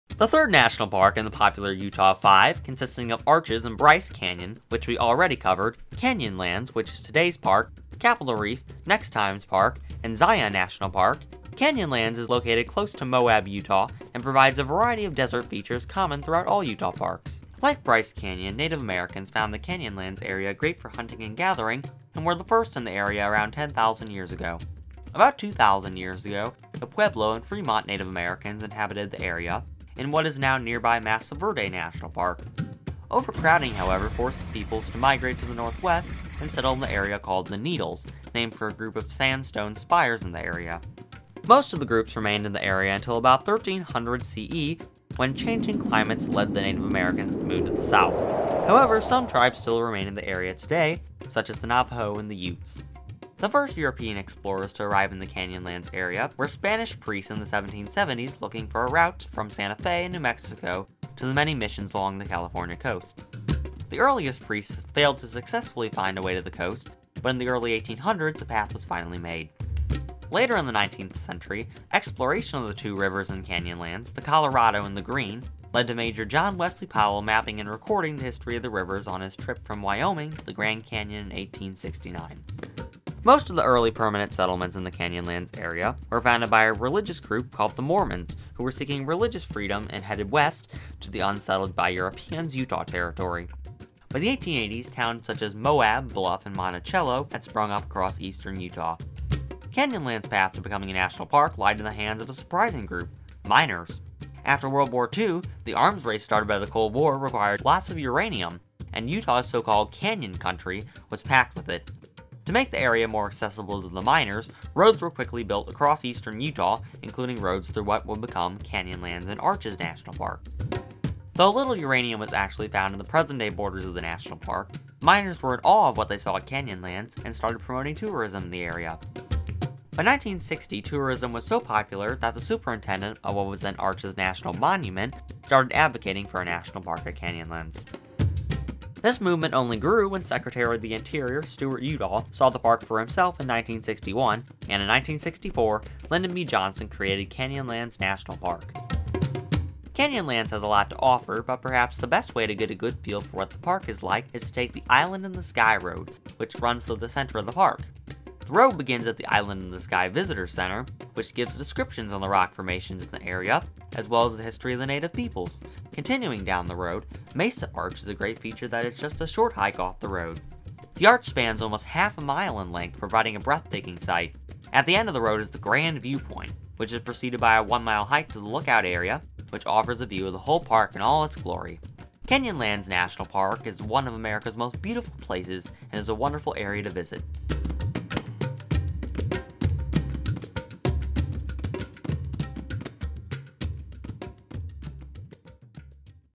Utah’s Canyonlands National Park
A wide plateau extends across the landscape of Canyonlands National Park, about an 40 minute drive southwest of Moab, Utah... and that's where our story begins. Today we take a look at some usual and unusual suspects that helped this park become what it is today, as well as some fun places within the park to visit and explore. This is wild Utah at its best.